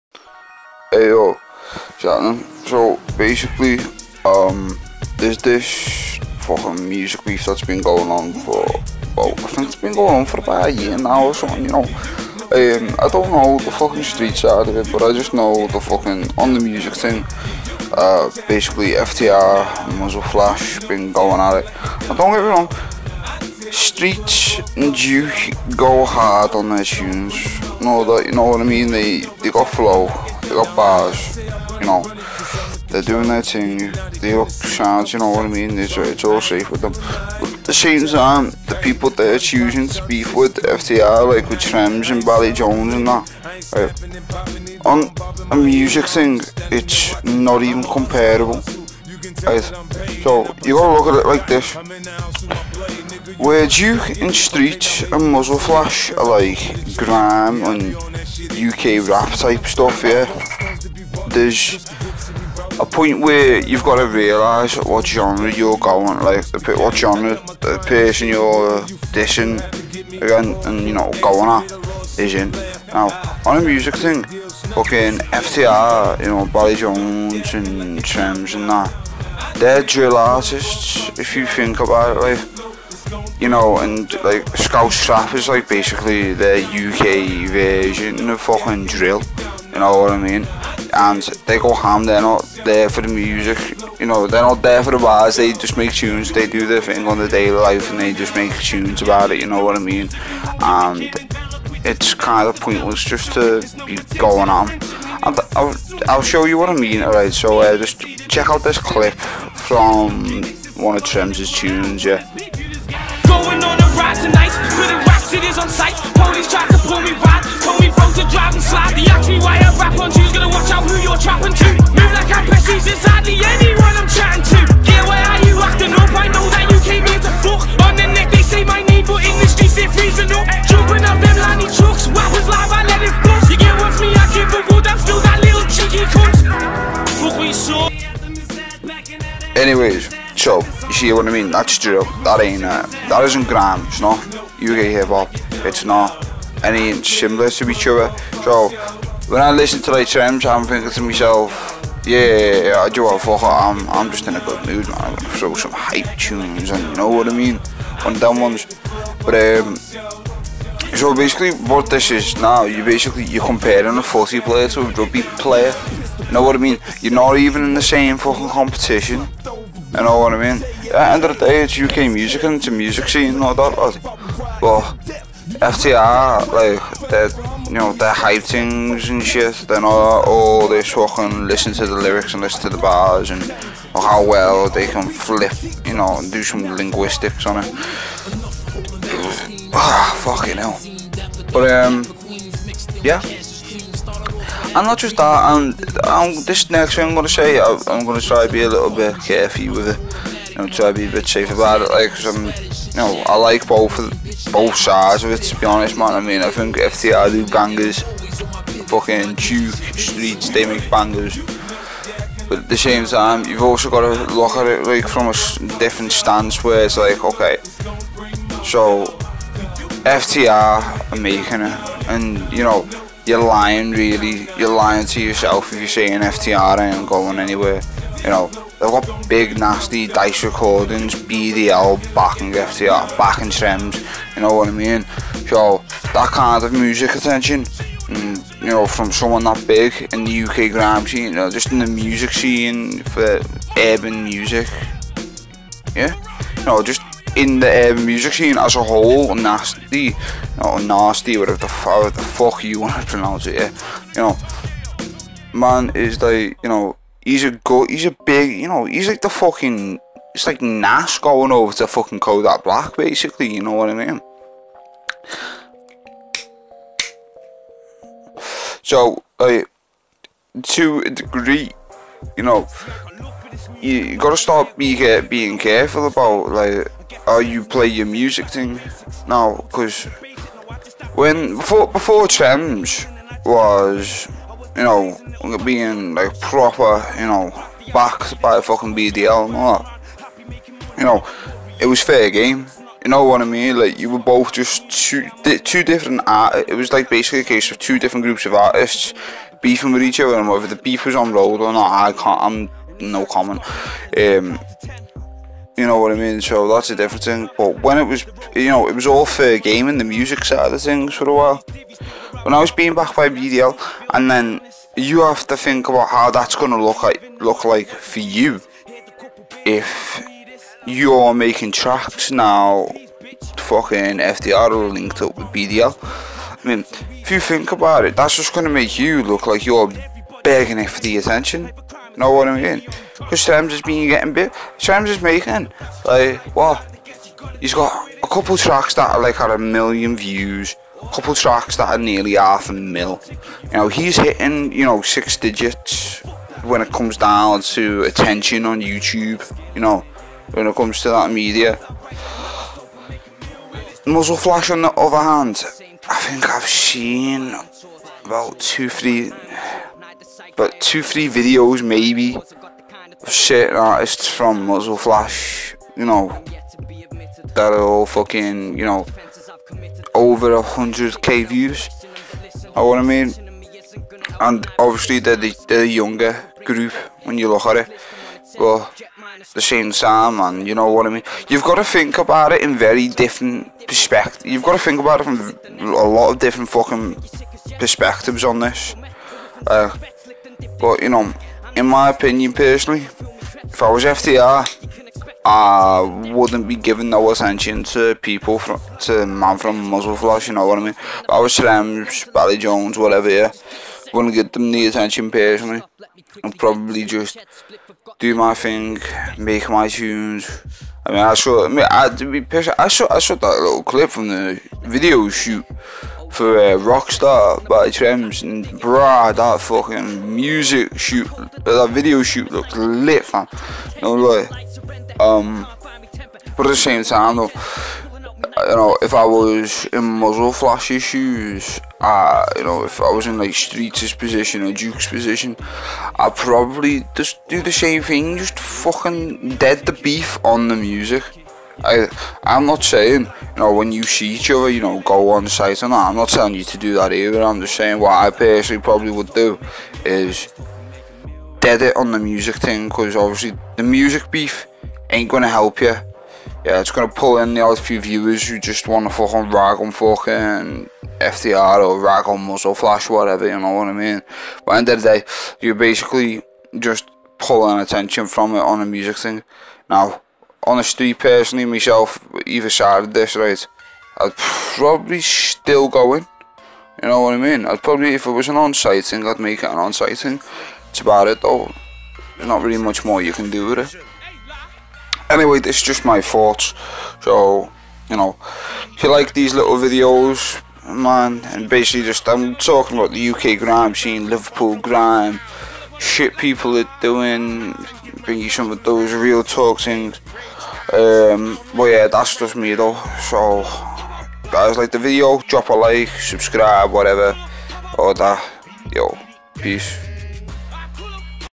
FTR vs MuzzleFlash Beef
Just my opinion on this whole beef ting with FTR and MuzzleFlash.